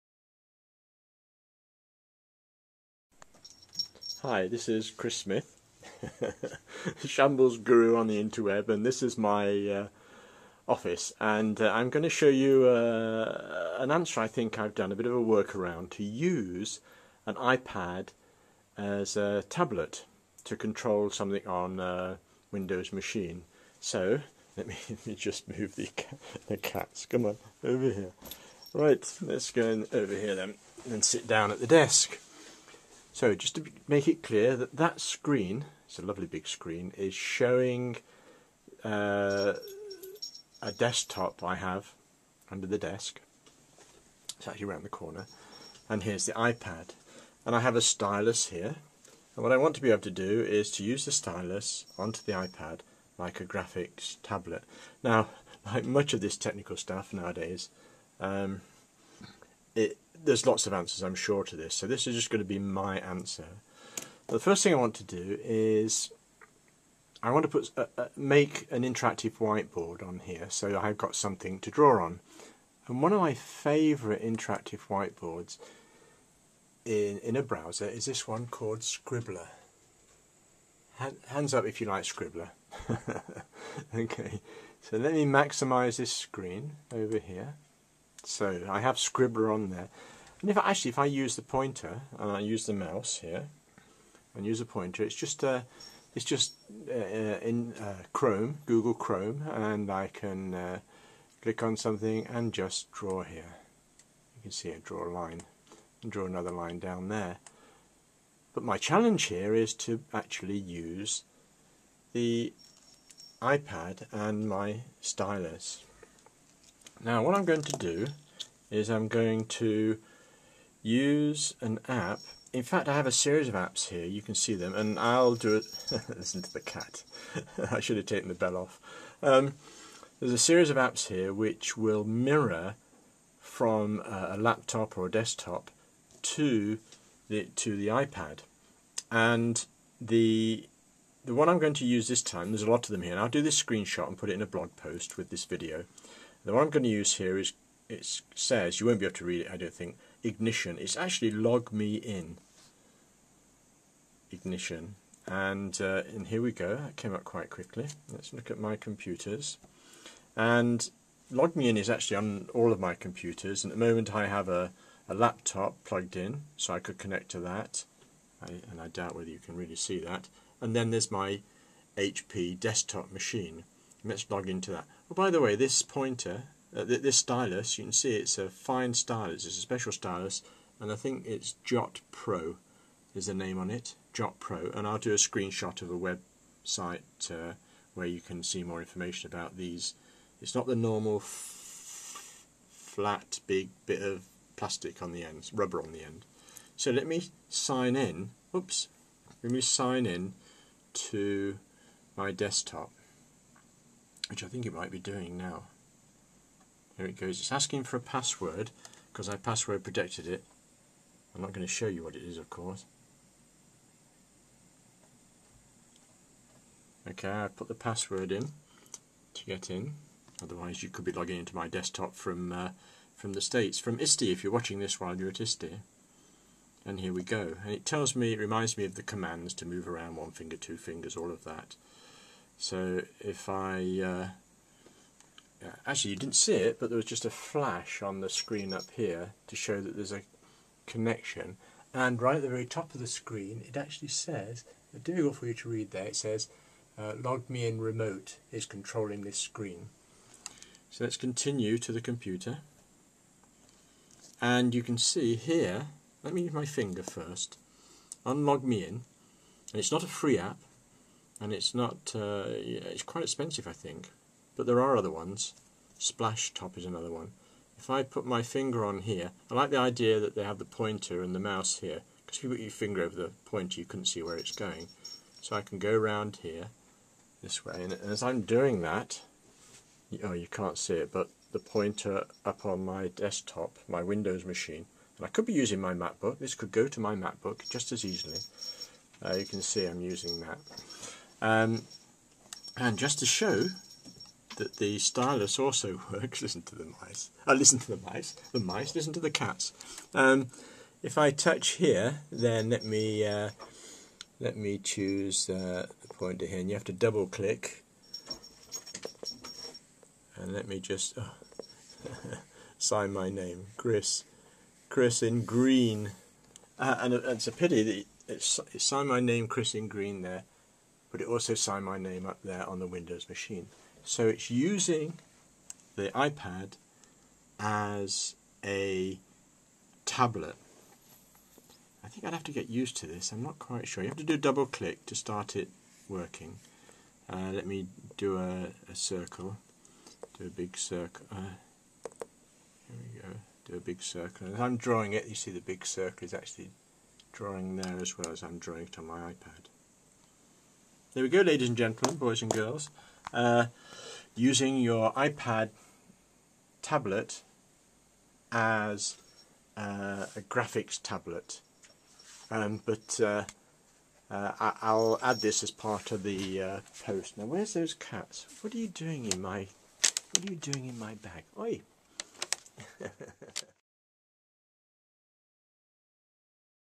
The iPad as a Graphics Tablet
A request in a Ning forum asked (June 2012) if an iPad could be used as a graphics tablet to control actons on a laptop or desktop machine where screencasts would be recorded. [thanks for the question Juliana]

Here's a quick attempt at answering that ... late one night with the help of my cats ;-)

It is also part of a more comprehensive Blog Post I did about Remote Apps for the iPad